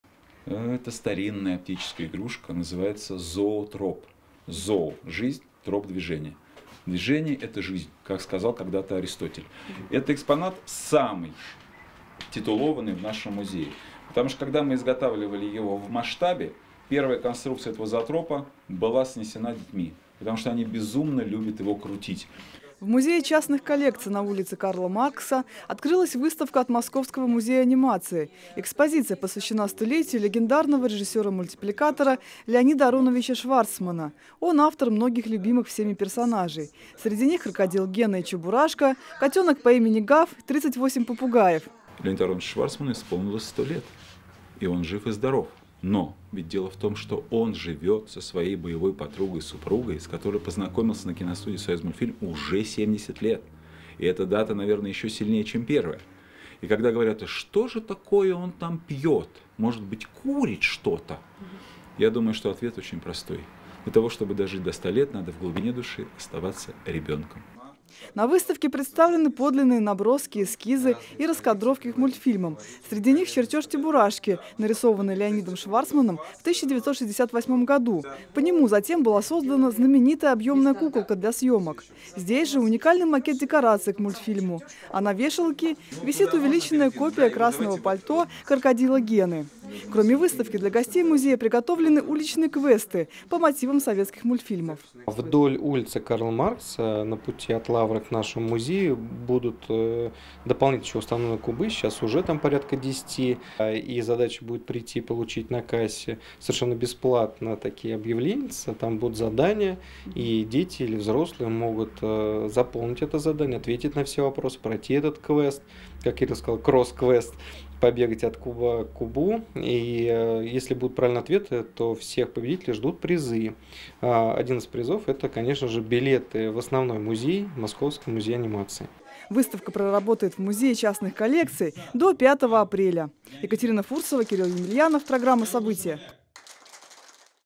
Он нарисовал Крокодила Гену и Чебурашку – выставка по случаю 100-летия Леонида Шварцмана
В Музее частных коллекций на улице Карла Маркса в Сергиевом Посаде открылась выставка Московского музея анимации.